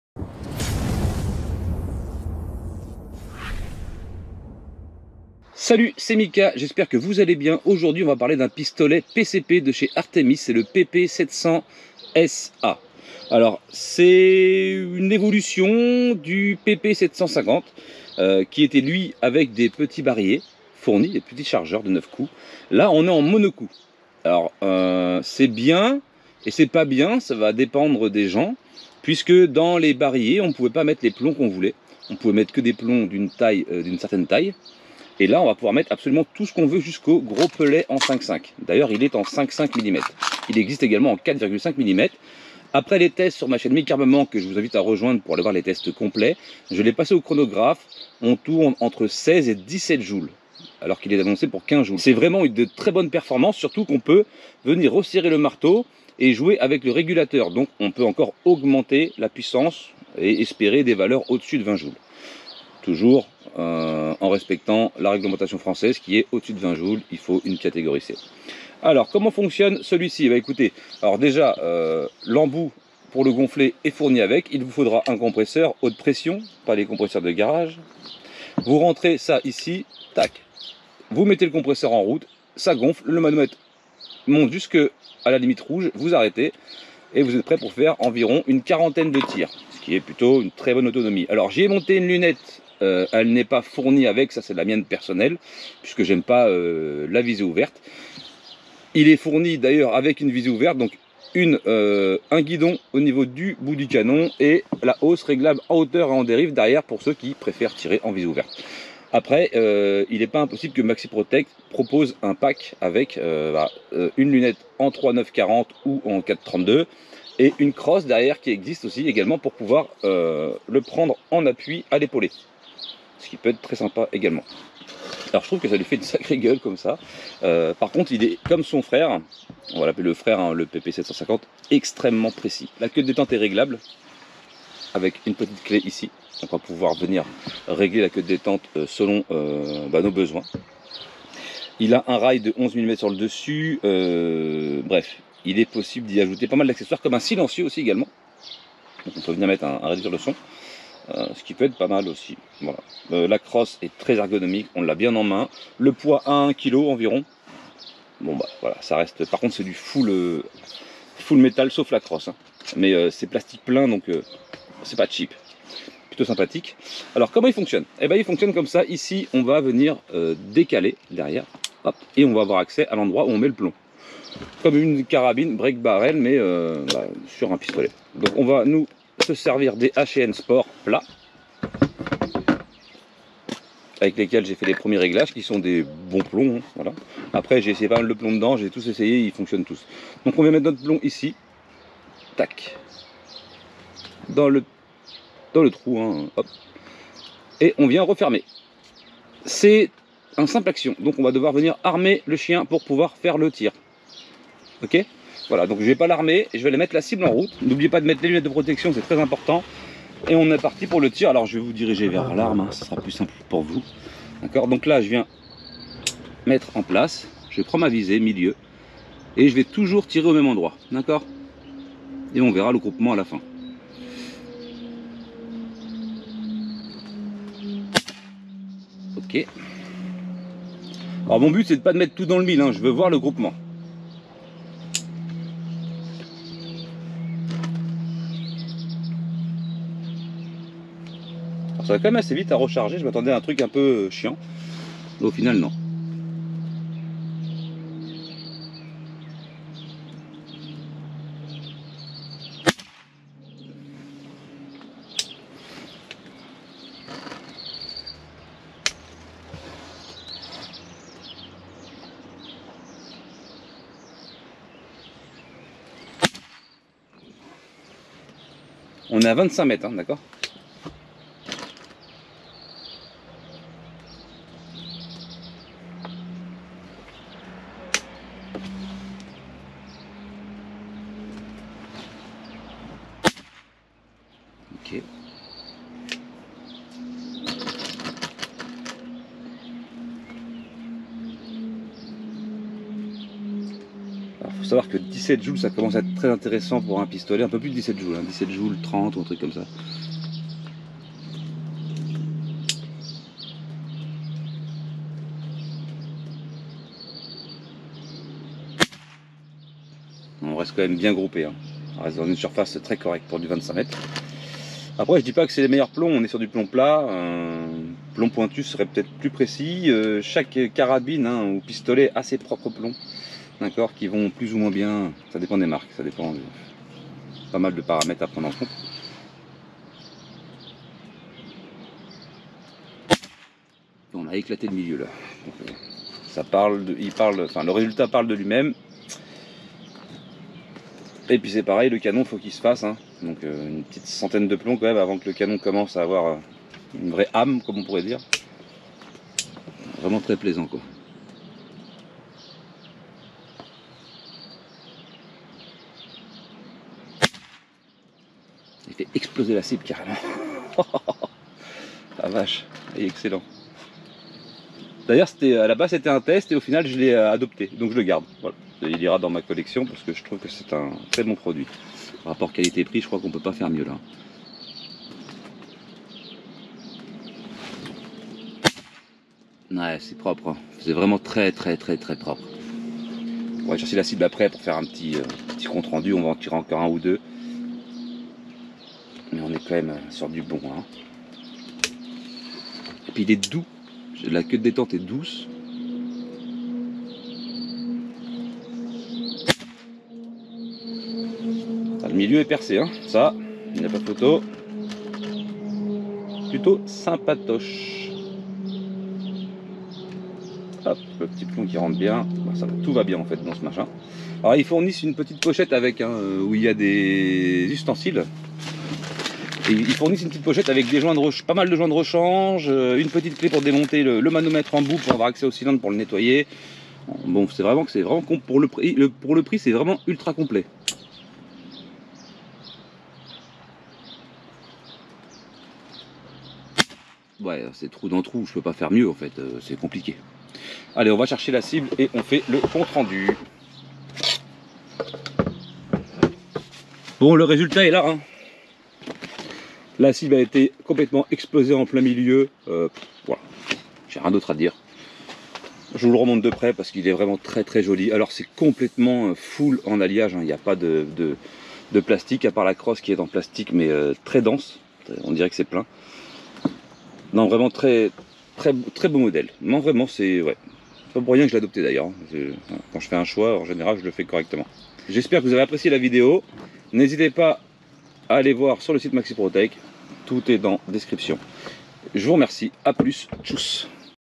Artemis PP700S-A 5.5mm : une capacité de tir de 50 plombs pour une puissance de 15 joules
✨ Découvrez le pistolet à plombs Artemis PP700S-A 5.5mm ✨

🌐 Pour tous les détails sur ce pistolet révolutionnaire ou pour l'acquérir, cliquez

🎯 Entrez dans l'univers de la précision avec le pistolet PCP Artemis PP700S-A. Conçu pour les amateurs de tir de loisir exigeants, ce modèle allie légèreté, maniabilité et performance à un prix accessible, se positionnant comme une excellente alternative aux pistolets à CO2. Sa capacité à tirer 50 plombs par charge avec une puissance de 15 joules vous promet une expérience de tir inégalée.

🕒 Points forts de notre vidéo :
- [0:10] Introduction au pistolet Artemis PP700S-A
- [1:00] Caractéristiques techniques et avantages
- [2:50] Guide pratique de chargement et réglages
- [3:40] Test complet de l'arme sur cibles
- [7:25] Résultats et conclusion du test

🔍 Caractéristiques marquantes du Artemis PP700S-A :
- Jusqu'à 50 plombs par charge
- Puissance de 15 joules 🚀
- Poids de 890 grammes
- Poignée ergonomique en ABS, adaptée aux droitiers comme aux gauchers
- Facilité de chargement : accès simplifié au port de chargement des plombs

Votre soutien via les commentaires 💬 et les likes 👍 nous est précieux pour continuer à vous proposer des contenus de qualité. Restez connectés pour plus de découvertes ! 😊